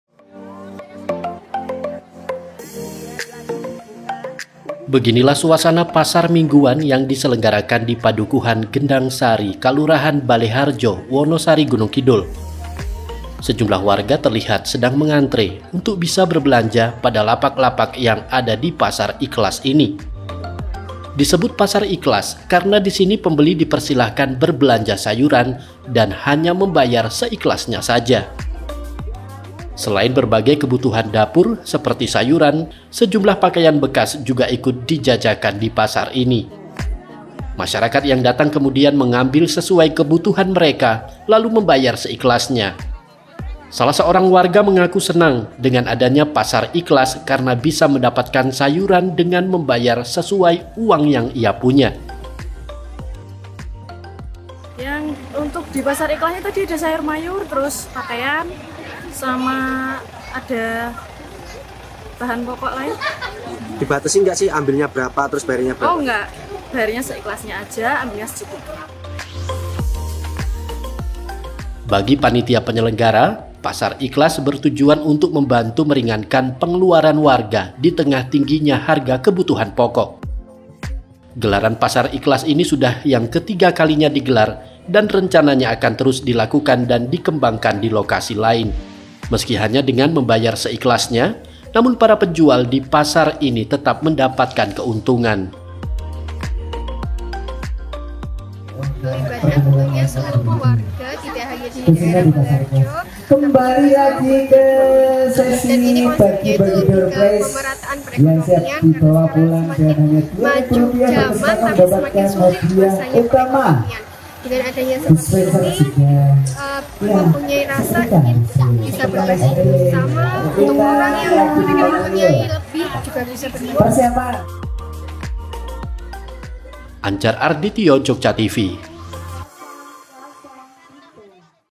PASAR SAYUR UNIK DENGAN KONSEP BAYAR SEIKHLASNYA
SEPUTARJOGJA-Di Kabupaten Gunungkidul, terdapat pasar unik dengan mengusung konsep bayar seikhlasnya. pasar ini diinisiasi kelompok UMKM di Kalurahan Baleharjo Wonosari, dimana pembeli dipersilahkan mengambil berbagai macam sayuran, dengan hanya membayar seikhlasnya saja.